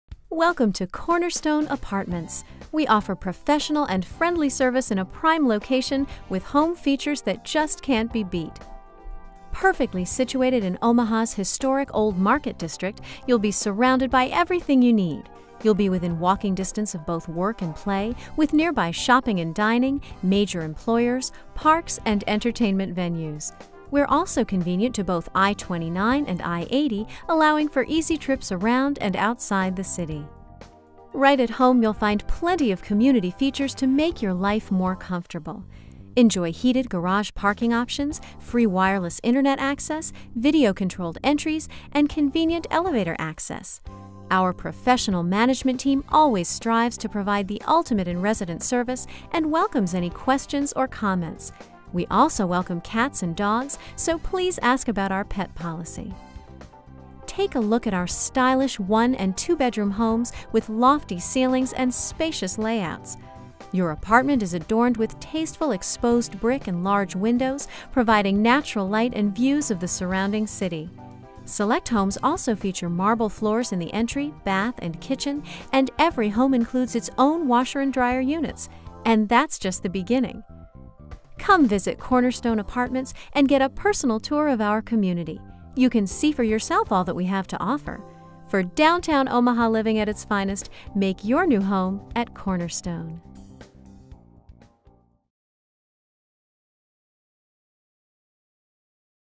Cornerstone Apartments in Omaha, NE - ForRent.com
Cornerstone Apartments located in Omaha, Nebraska's beautiful historic Old Market district provides downtown living at its finest. You'll love our spacious one bedroom and two bedroom apartment homes which offer high ceilings, large windows with abundant amounts of natural light, charming exposed brick, marble entry, bath & kitchen floors (in select units), washer and dryer, FREE wireless internet access, elevator access, video controlled entries and your dog or cat is welcome too (call for details)! There is never a shortage of parking at Cornerstone because we provide our residents with a heated parking garage for $95 a month. Our spectacular location allows you to walk to the Qwest Center, Holland Performing Arts Center, Heartland of America Park, the Old Market, major employers, fine dining, shopping, entertainment and allows for easy access to I-29 and I-80! Call today to set up an appointment to see your next apartment home at Cornerstone Apartments in Omaha, Nebraska's historic Old Market District!